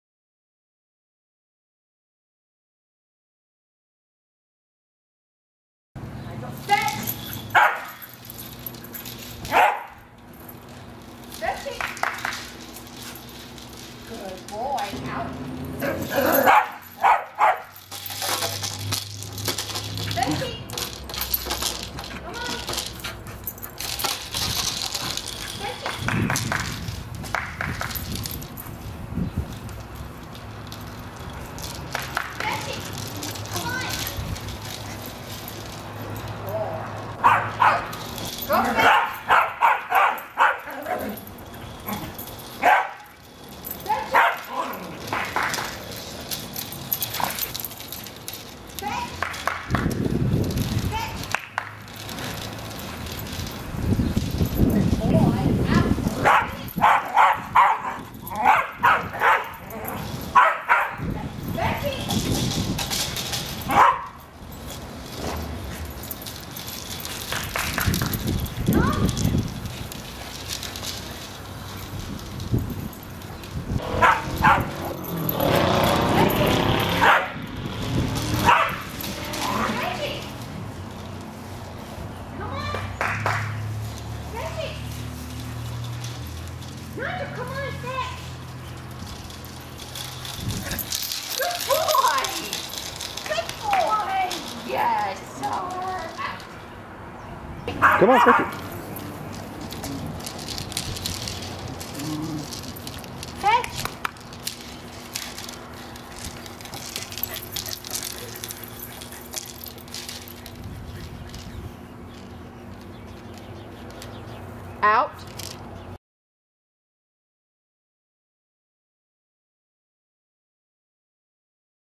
Welsh Terrier Nigel vs. the Hula Hoop
Like many dogs, Nigel was obsessed with large round rolling objects. He was driven completely mental by a bowling ball in his backyard, At a dog show once he attacked the wheels of a mans wheelchair. Fortunately the man was an old hand with Welsh Terriers and found the whole episode hilarious.

Here we see Nigel very excited by the Hula Hoop. He dealt with it in his typically unique fashion.

R.I.P. Ch. Making Plans For Nigel J.E.